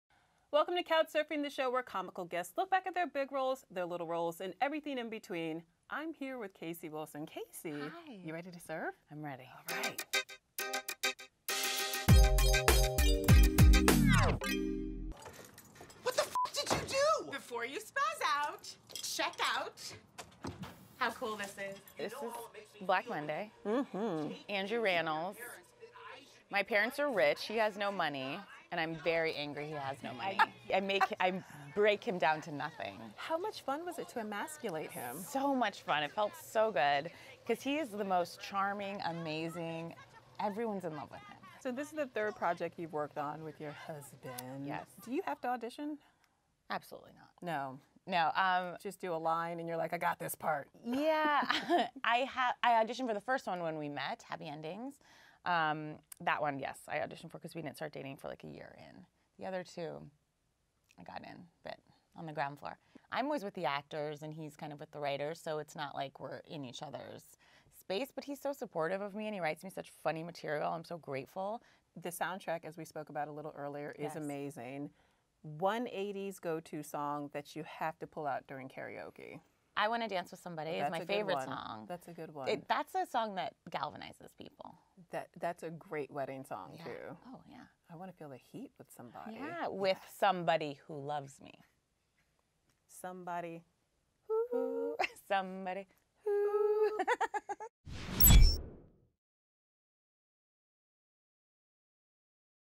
Casey Wilson On Her On-screen BF Andrew Rannells In ‘Black Monday’ | PeopleTV | Entertainment Weekly
Casey Wilson talks about her new Showtime show, 'Black Monday' and working with Andrew Rannells.